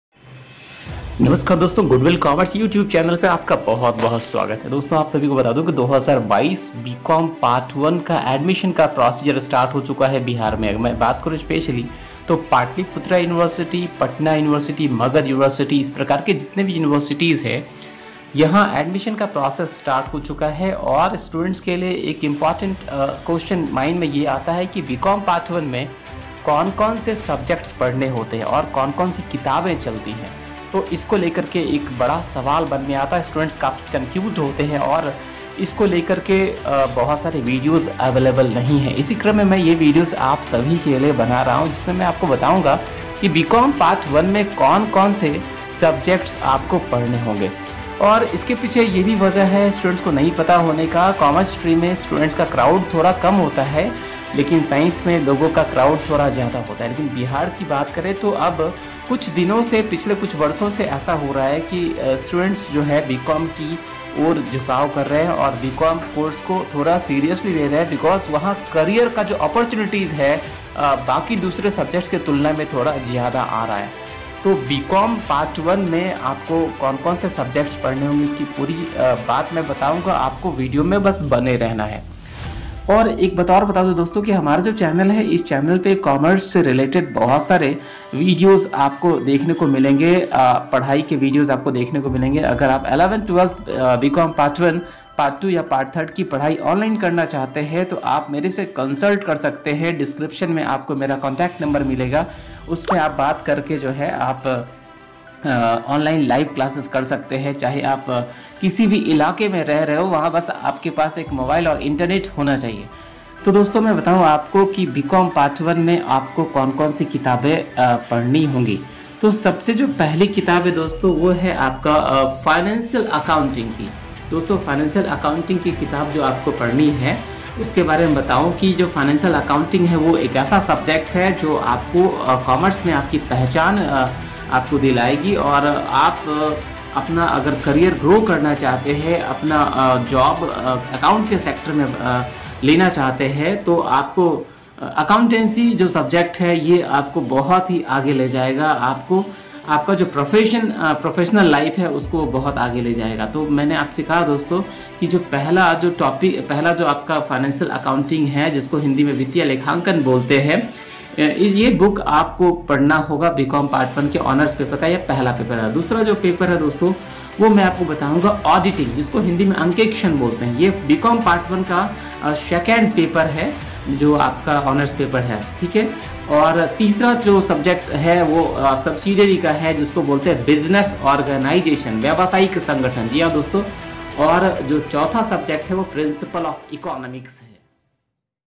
पाटलिपुत्र यूनिवर्सिटी पटना बीकॉम पार्ट 1 में कौन-कौन सी किताबें पढ़नी है 2022-23
PPU B.com Part 1 Books 2022-23
financial accounting
auditing
business organisation
principles of economics
PPU Accounts Hons 2022-23